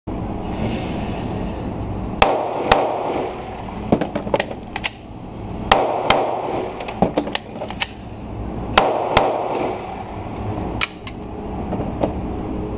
2 x 2 x 2 vp9 9mm Drill
Reload Drill...trying to get accustomed to the mag release on the trigger guard; Patrick Utah Coleman